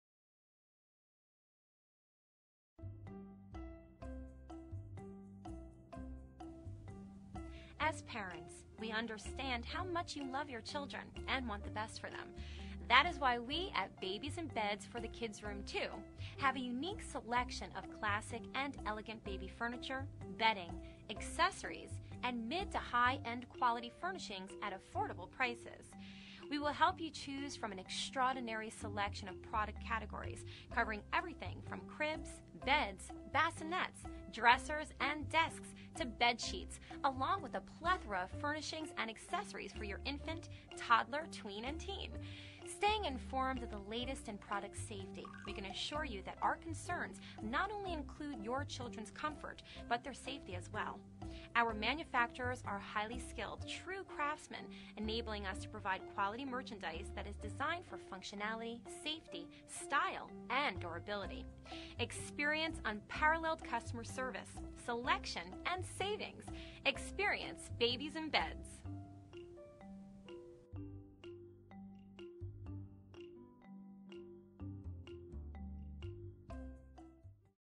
Corporate Video - Childrens Furniture -Babies'n Beds - OMG National - Florida
Babies can be a handful, take some of the stress of shopping for your baby by visiting Babies 'n Beds. Their customer friendly service and vast selection of products will make any purchase easy for you so you can get back to what matters most.